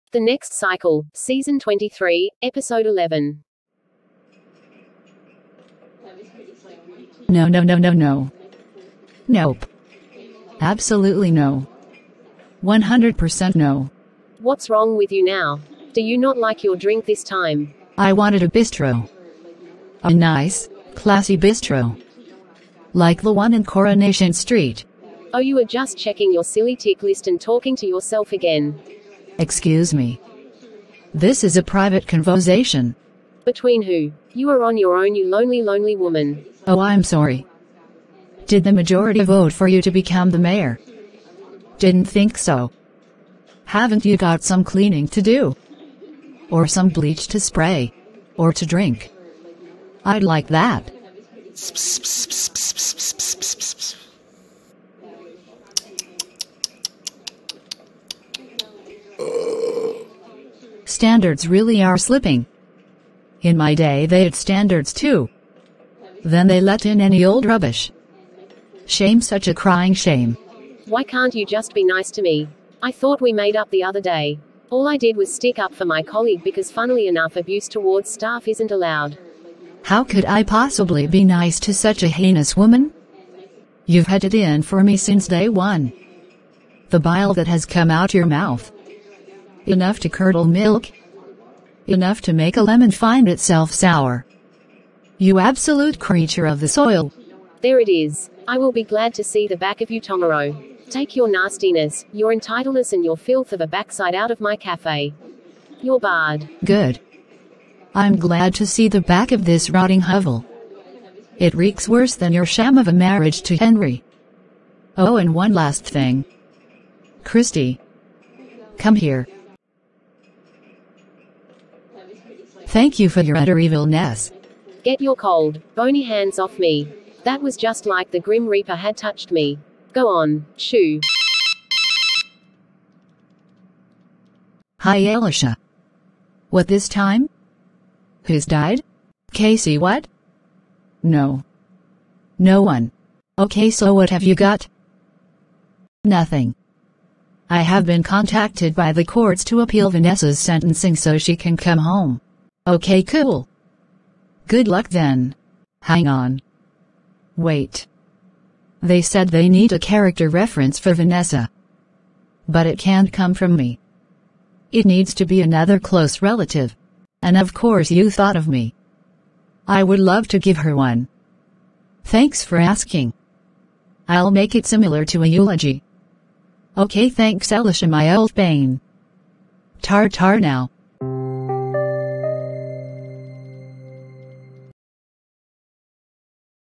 The Next Cycle: Season 23: Episode 11: The Last Arguement:
Nothing lasts forever. That's why Christi and Deborah have their final showdown.

(I do not own Plotagon and every story I have created has been from my own imagination, I have created this for fun.) Hope you viewers enjoy!